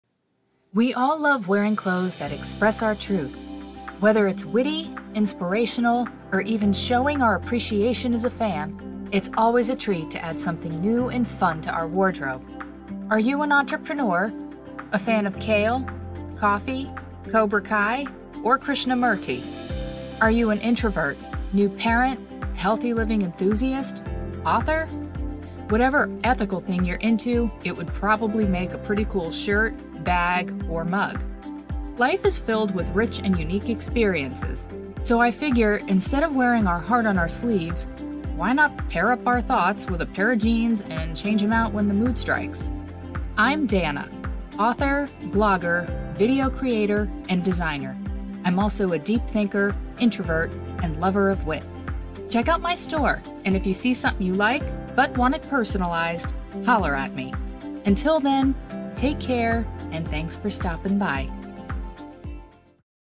My Etsy Shop
This is an example of what you can create for your Etsy store.

It isn't a tutorial...but I did use Doodly whiteboard explainer video animation, did my own voiceover and added images.

This just goes to show you can be as creative as you wish.
If you'd like to see a tutorial of how I made this, let me know int he comments.